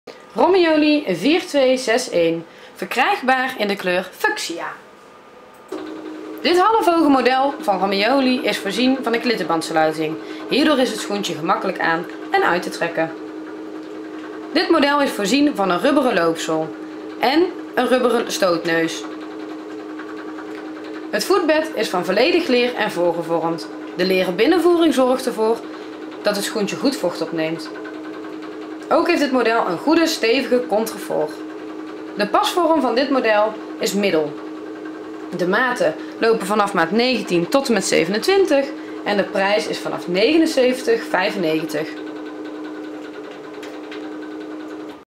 Romagnoli kinderschoenen 4261
Kleuren

Deze kinderschoenen zijn verkrijgbaar in de kleuren Fuchsia, Turquoise, Goud en Roze.

Pluspunten

– De kinderschoenen zijn voorzien van een goede stevige contrefort zodat de kinderen die deze schoenen dragen mooi recht op de schoenen lopen.
– De kinderschoenen zijn voorzien van echt EVA rubberen zolen die erg slijtvast zijn.
– Daarnaast zit er een goede stevige stootneus op de 4261 van Romagnoli.
– De kinderschoenen zijn voorzien van een goed voorgevormd leer (leder) voetbed dat mooi naar de voet vormt en goed vocht opneemt.

Sluiting

De kinderschoenen zijn voorzien van een klittenband sluiting. De klittenband is enorm handig omdat de schoenen vaak zelfstandig door het kind aan en uit gedaan kunnen worden.

Breedte

Daarnaast is de 4261 van Romagnoli voorzien van een mooie gemiddelde pasvorm.

Maten en prijs

Verkrijgbaar in maat 19 t/m 27
Kosten vanaf : €79,95